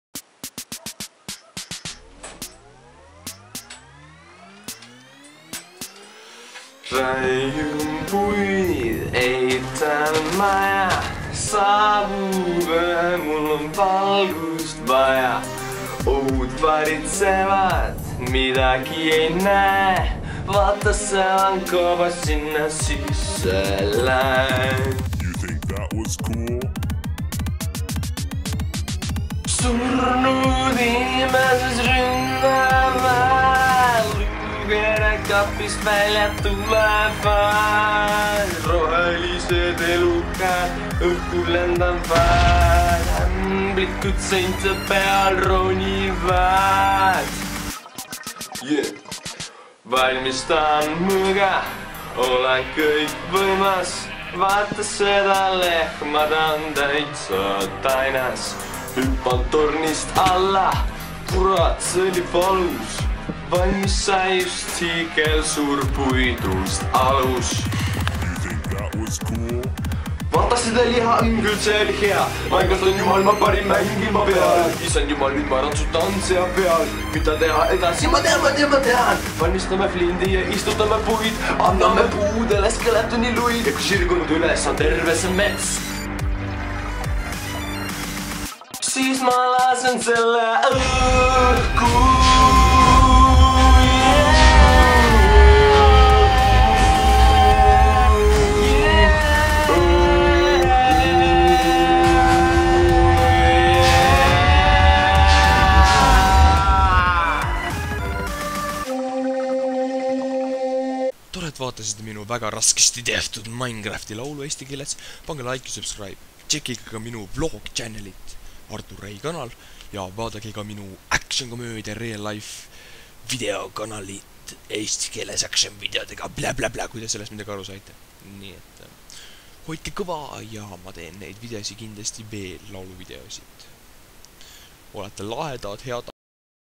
Minecrafti laul-Eesti keeles!!!(räpp ka)
Muusika tegin:Fl-studio.laulsin ise.
Suur-suur tänu,et vaatasite seda laulu,ma nägin sellega palju-palju vaeva :) kui teile meeldib,siis teen selliseid muusikavideosi veel :) sest see tuleb mul isegi välja :)

JÄRGMISE EPISOODINI,
HOIDKE KÕVA!